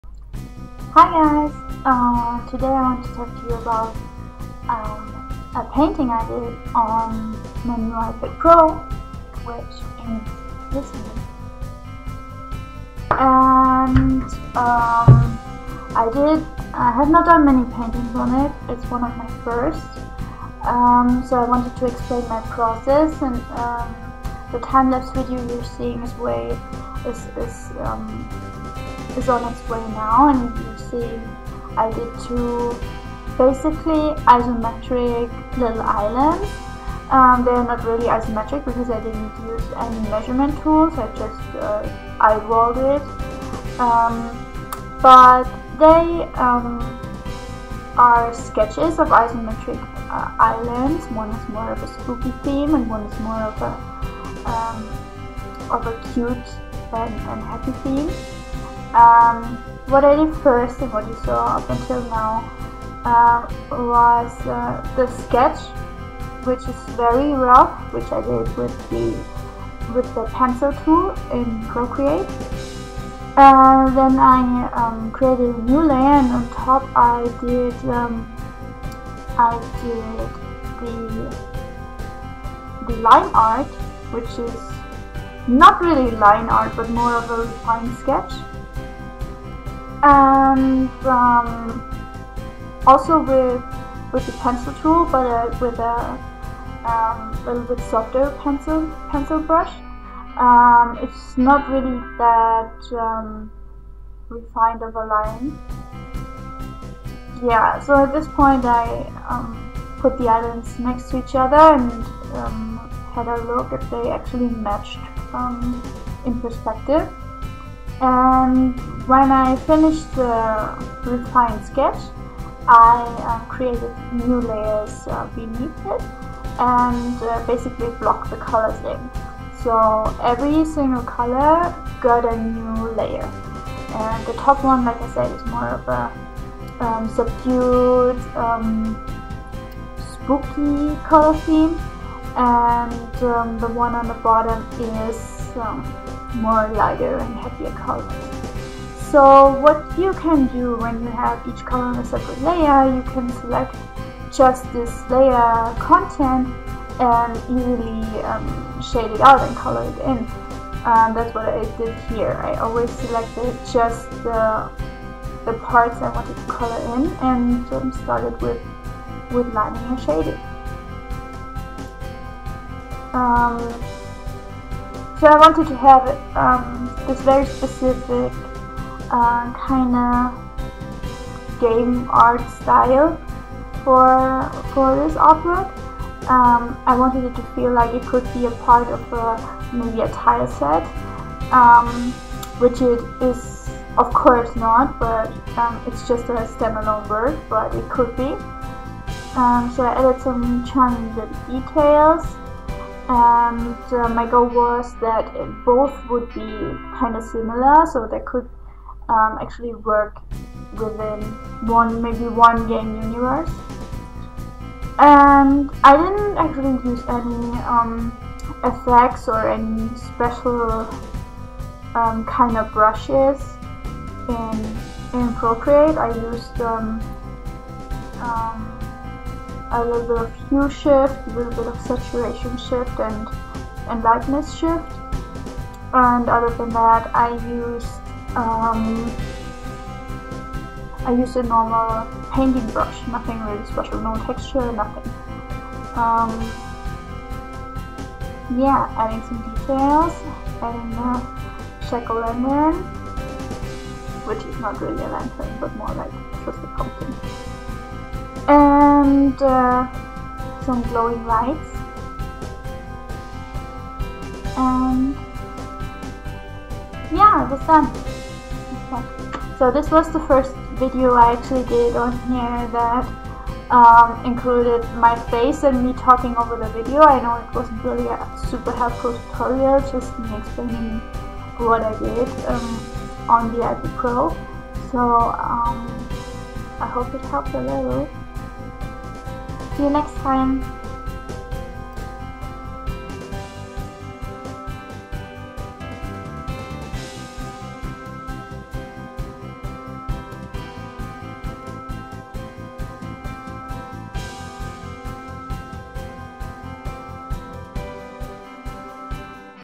Painting on the iPad Pro: Isometric Game Environment // HALLOWEEN
This is a timelapse video of a painting I did in Procreate on the iPad Pro! I know the sound quality isn't great, will try to improve it if anybody is interested in seeing more videos like this one.
Also in the end my cat Babu makes a guest appearance which is probably the highlight of this video.